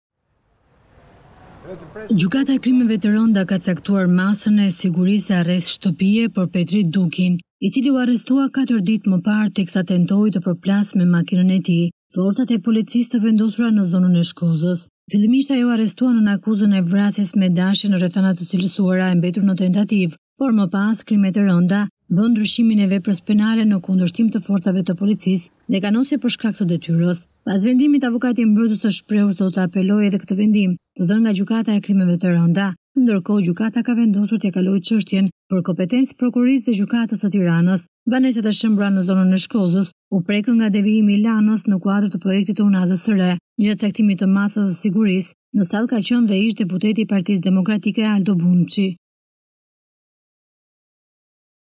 Ora News - Tentoi të përplasë me makinë forcat e policisë, "arrest shtëpie" për banorin e Shkozës
Gjykata për Krime të Rënda ka caktuar masën e sigurisë arrest shtëpie për Petrit Dukin, i cili u arrestua 4 ditë më parë teksa tentoi të përplasë me makinën e tij forcat e policisë, të vendosura në zonën e Shkozës.

Fillimisht ai u arrestua nën akuzën e vrasjes me dashje në rrethana të cilësuara e mbetur në tentativë, por më pas Krimet e Rënda bënë ndryshimin e veprës penale në kundërshtim të forcave të policisë dhe kanosje për shkak të detyrës.

Pas vendimit, avokati mbrojtës është shprehur se do ta apelojë edhe këtë vendim të dhënë nga Gjykata e Krimeve të Rënda.

Ndërkohë gjykata ka vendosur tja kalojë çështjen për kompetencë Prokurorisë dhe Gjykatës së Tiranës.

Banesat e shembura të Shkozës u prekën nga devijimi i Lanës në kuadër të projektit të unazës së re.